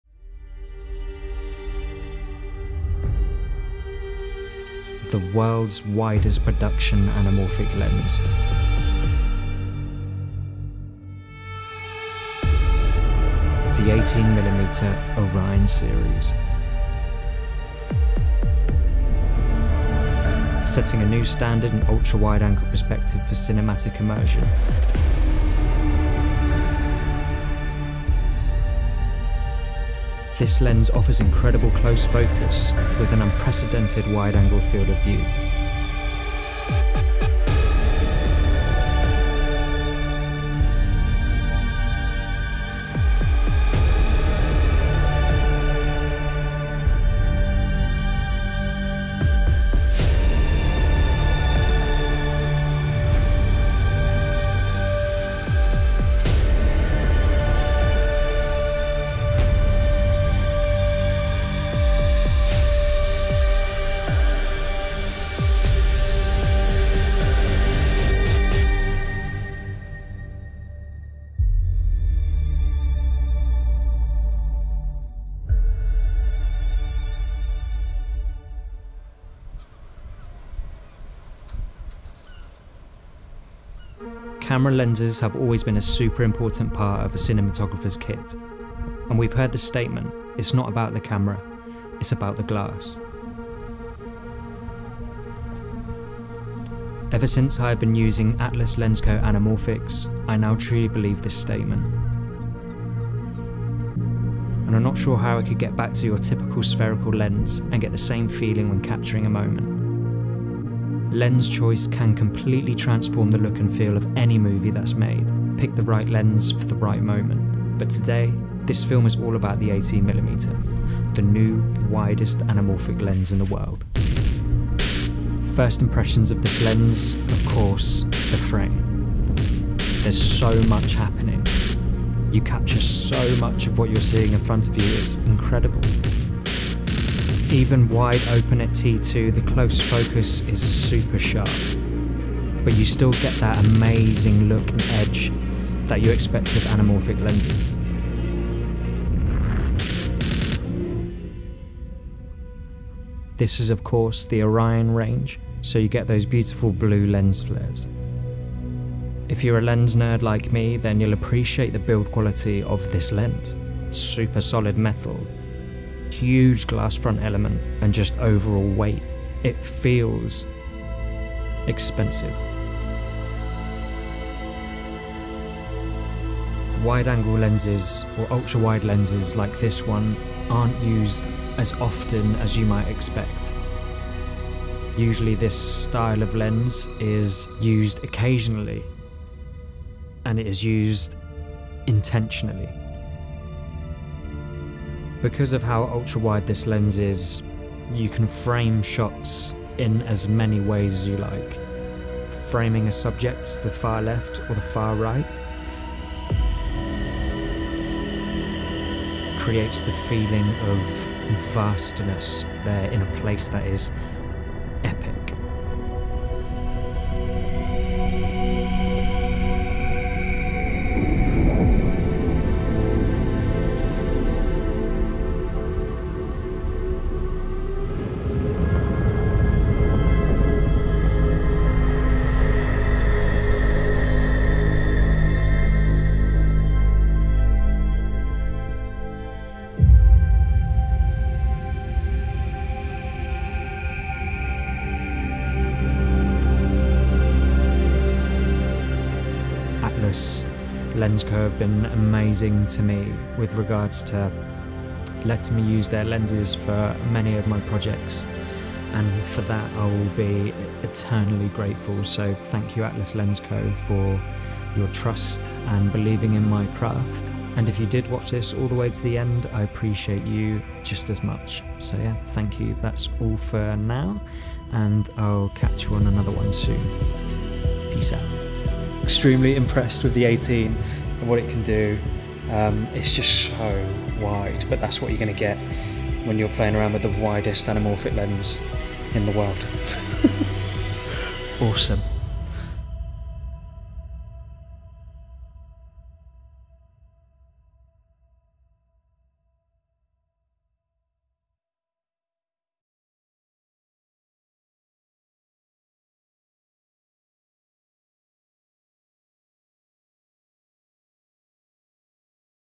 The Widest Anamorphic Lens In The World - Atlas Orion 18mm
Was very kindly trusted to test the brand new @AtlasLensCo 18mm Orion series lens the widest anamorphic lens in the world. Would have loved to have had more time with the amazing lens. But enjoyed shooting what I did in the time frame that I had.

The 18mm Orion Series is the world’s widest production anamorphic lens - setting a new standard in ultra wide angle perspective for cinematic immersion. This lens offers incredible close focus with an unprecedented wide angle field of view.

Shot on the @reddigitalcinema KOMODO 6K with the Sony FX3 on behind the scenes.

Coloured with @Dehancer

Music licenses

MB01VTJCZOUHZYH
MB01DXHUTDEILOE
MB01V05K1KWOFQA